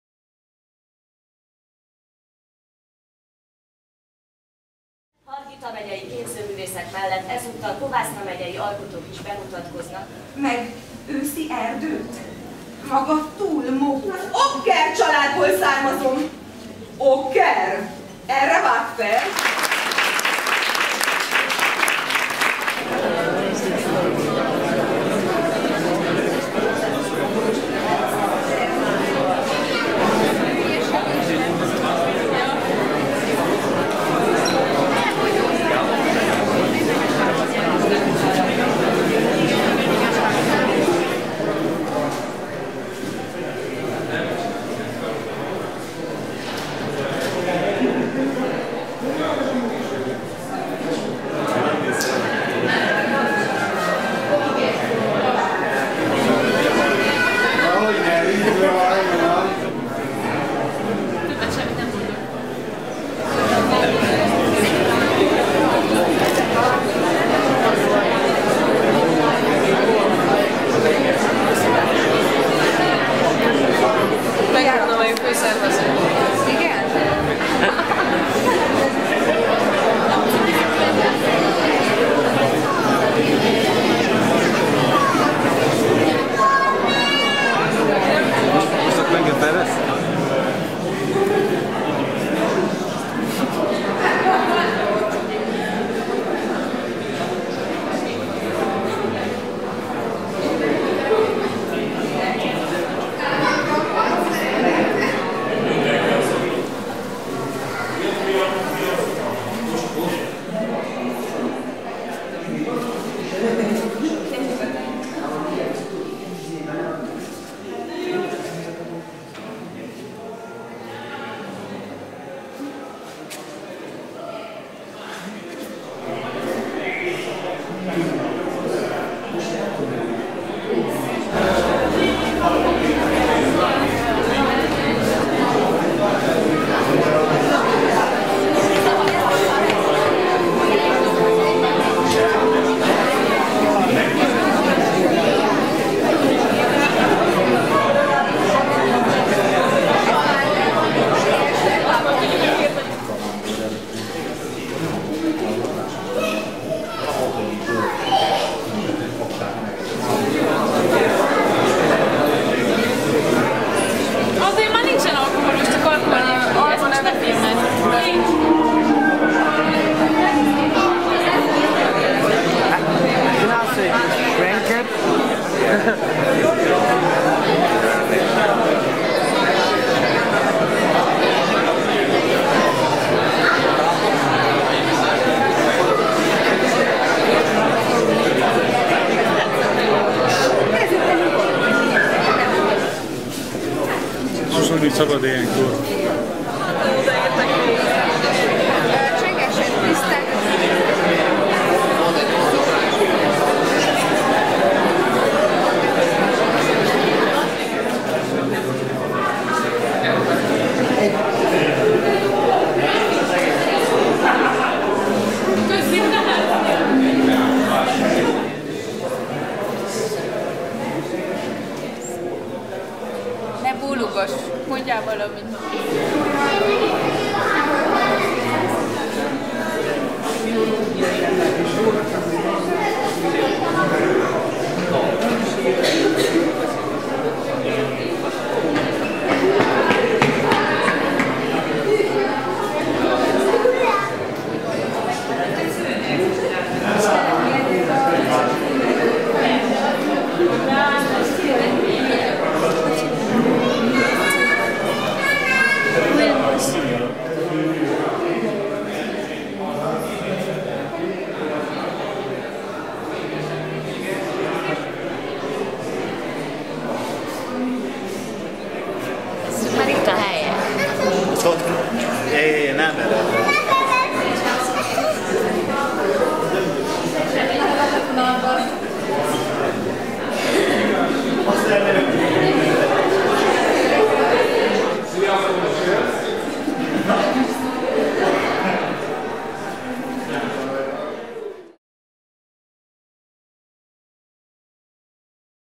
Projekt 2, Csíkszereda 2009
Contemporary Art Exhibition, Csíkszereda 2009

Organised by / Szerveztők:
Erdélyi Összművészeti Egyesület és Csík Terület Ifjúsági Tanácsa

Szülőföld Alap, Communitas Alapítvány, Eurotrans Alapítvány, Hargita Megye Tanácsa, Web Media Sound